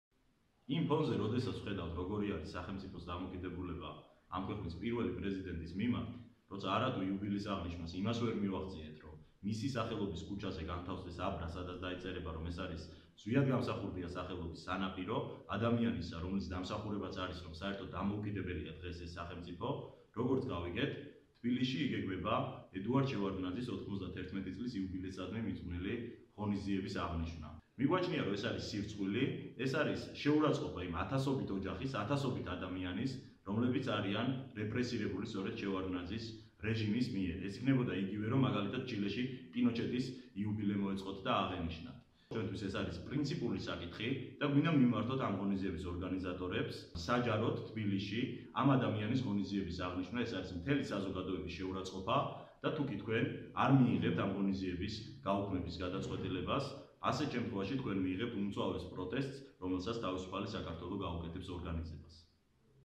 თენგიზ ომანიძე: შევარდნაძის იუბილესადმი მიძღვნილი ღონისძიება უნდა გაუქმდეს
თენგიზ ომანიძე, "თავისუფალი საქართველოს" თავმჯდომარის მოადგილე